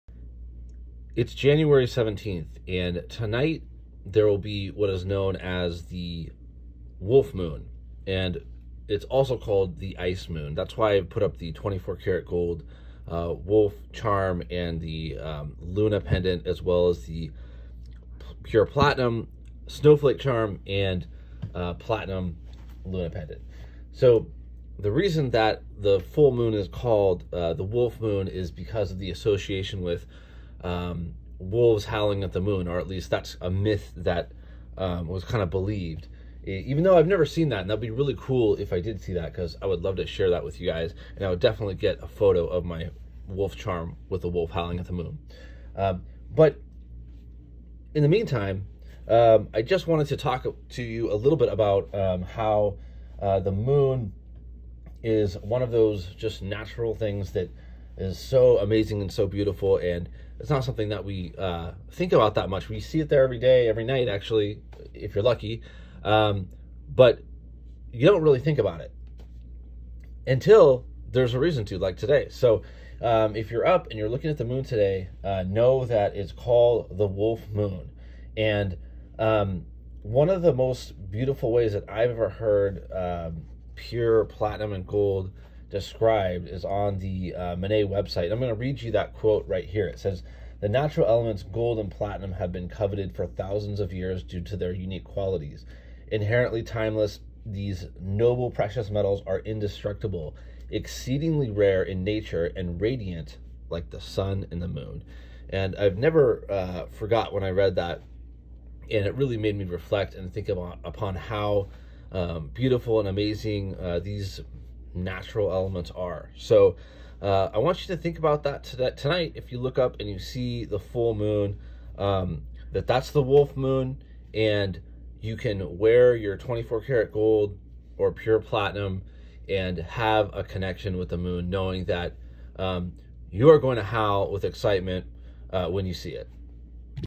24k Gold And the Wolf Moon
Tonight is the Wolf Moon.

“The natural elements Gold (Au) and Platinum (Pt) have been coveted for thousands of years due to their unique qualities. Inherently timeless, these noble precious metals are indestructible, exceedingly rare in nature, and radiant like the sun and the moon.”

Earn $5 in Mene credit for signing up for Mene with this invite

For more Mene jewelry and Totenpass close ups in real world scenarios see Goldmatters instagram